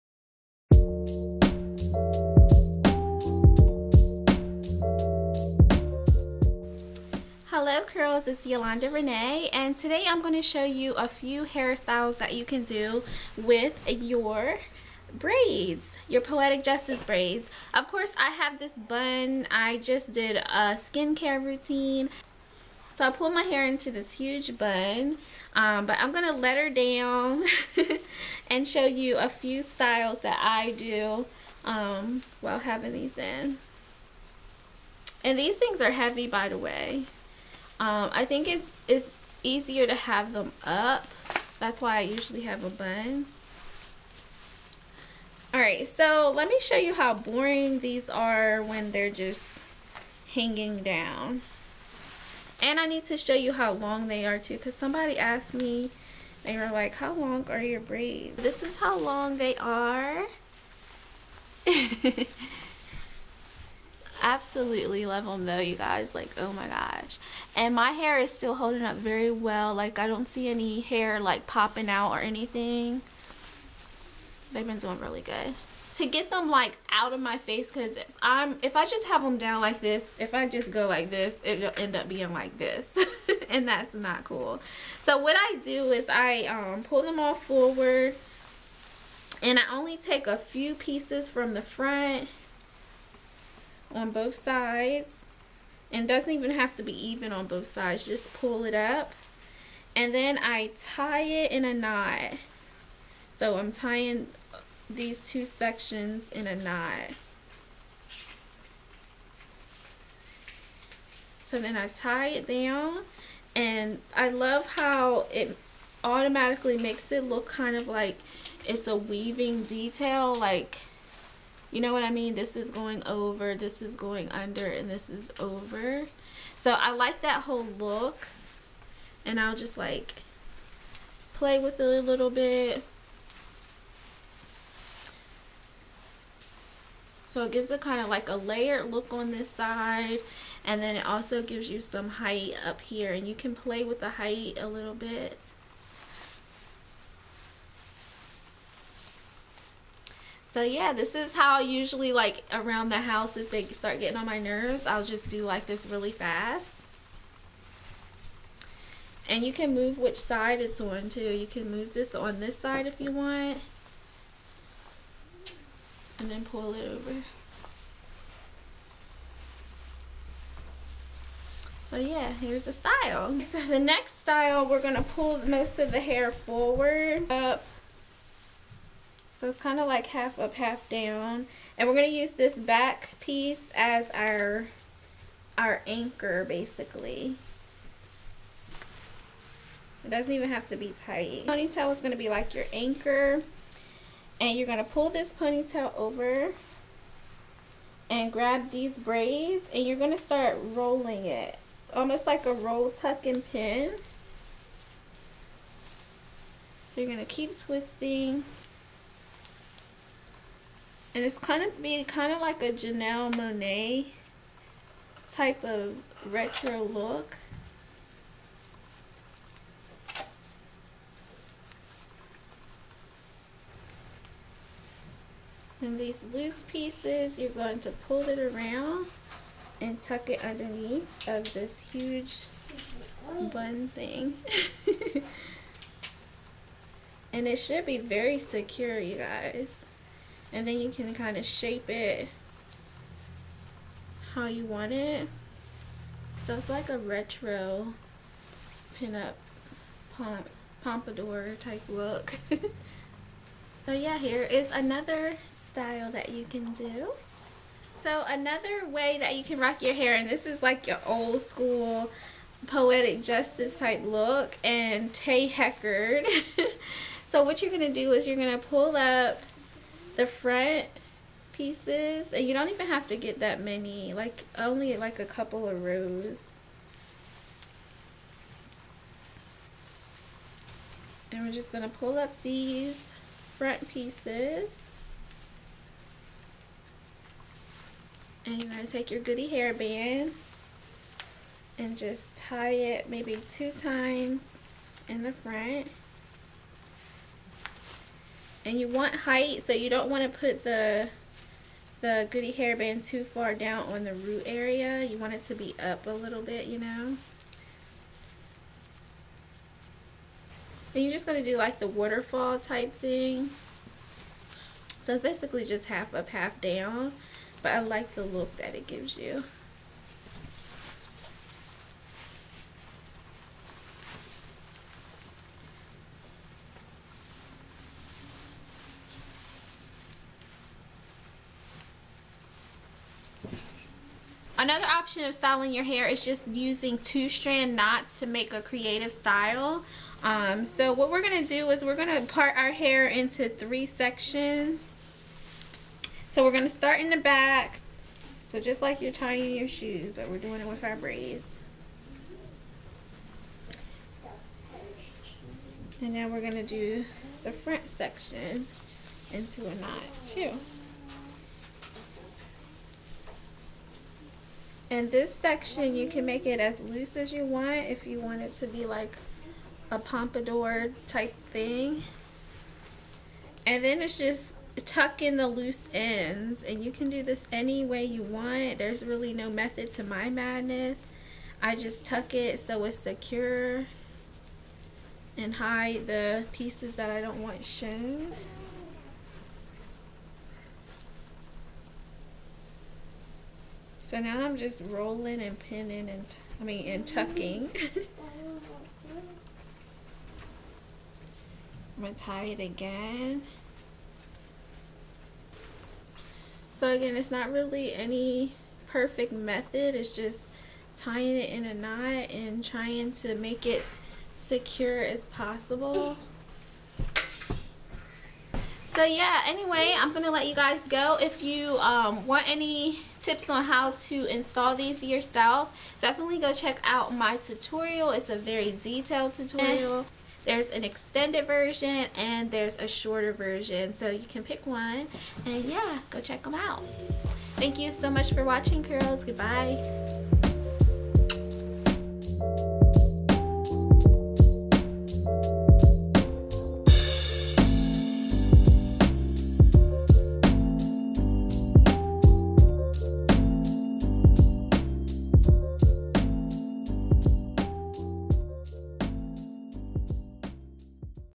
NATURAL HAIR | Quick Styles with Poetic Justice Braids (Box Braids)
I have permission for the use of the song/track in this video.

In this video I show you a few quick styles that you can do with your box braids. All you need is a goody hair band.